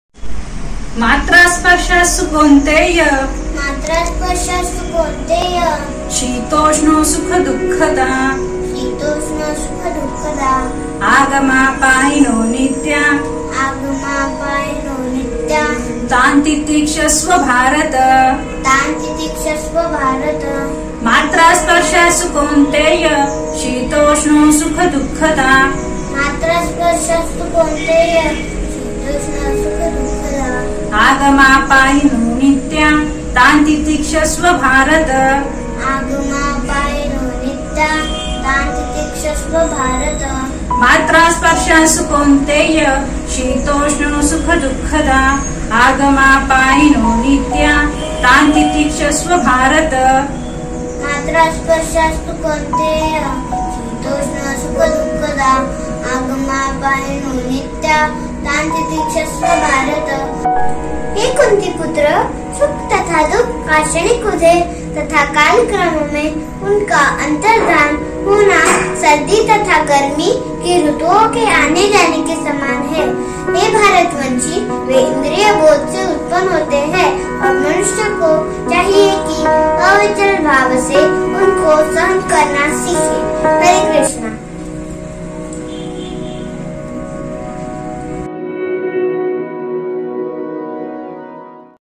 Shloka Recitation BG 2.14 at ISKCON Aurangabad Gopal Fun School
मात्रास्पर्शास्तु कौन्तेय शीतोष्णसुखदुःखदाः ।